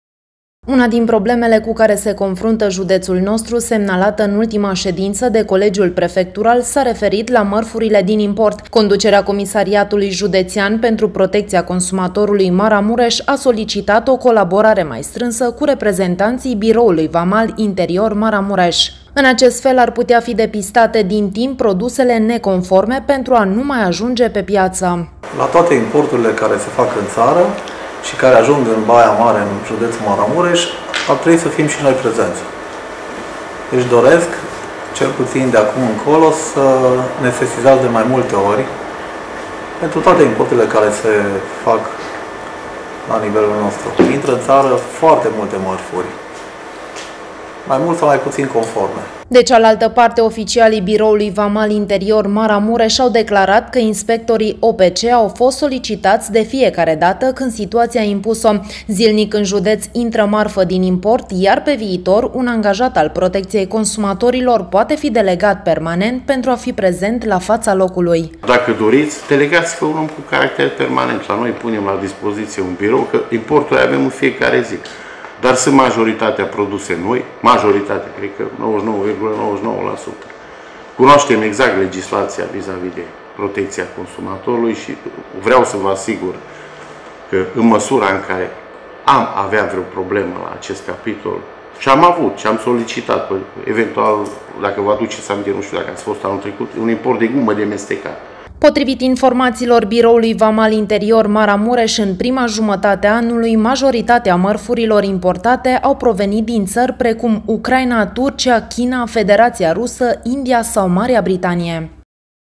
MĂRFURILE DIN IMPORT, ÎN ATENȚIA PROTECȚIEI CONSUMATORILOR
Mărfurile din import care intră în județ urmează să fie mai bine verificate pe viitor, pentru o mai bună protecție a consumatorilor. În ultima ședință de Colegiu Prefectural, instituțiile abilitate au căzut de acord că este nevoie de o colaborare mai strânsă.